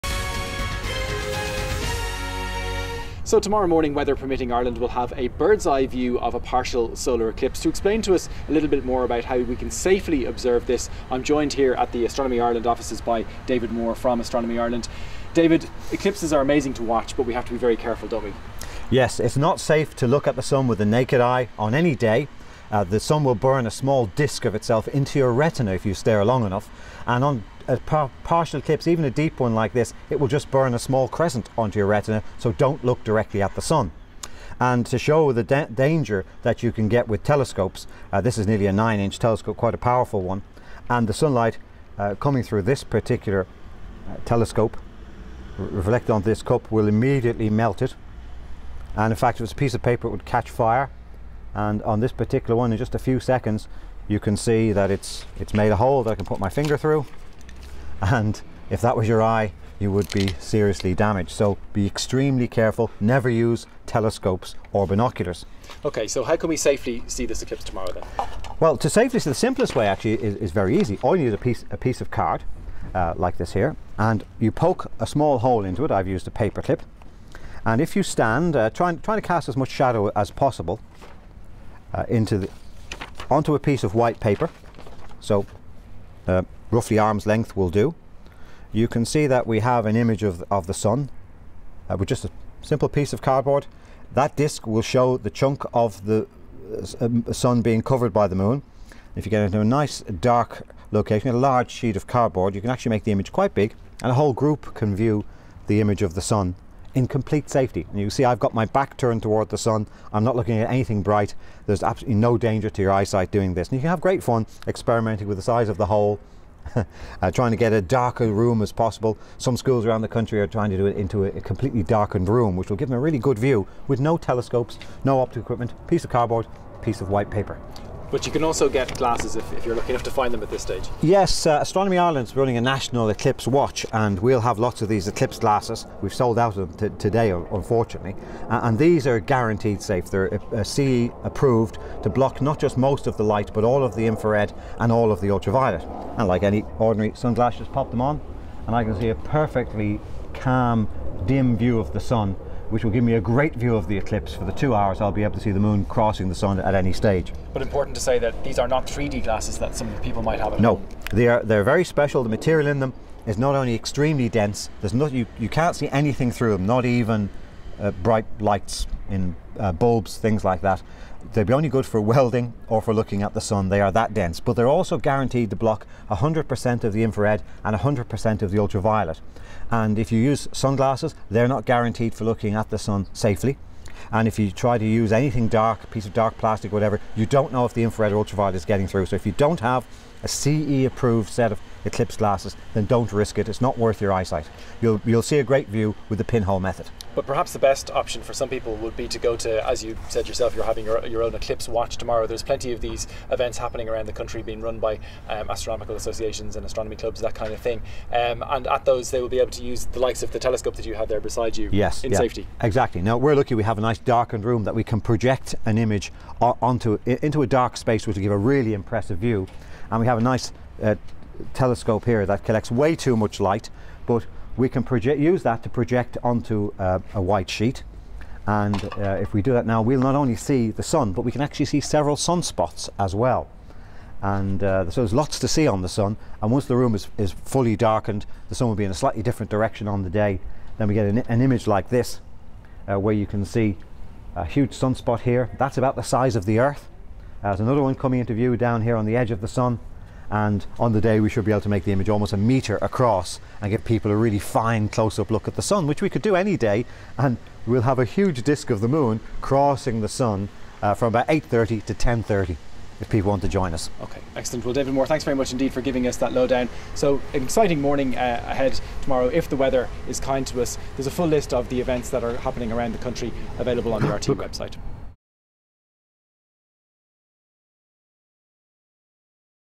How to view tomorrow's solar eclipse
Tomorrow morning between 8.30am and 10.30am, weather permitting, Ireland will have a perfect view of a near total solar eclipse.

RTÉ's Science and Technology Correspondent Will Goodbody speaks to David Moore of Astronomy Ireland about how best to observe the eclipse.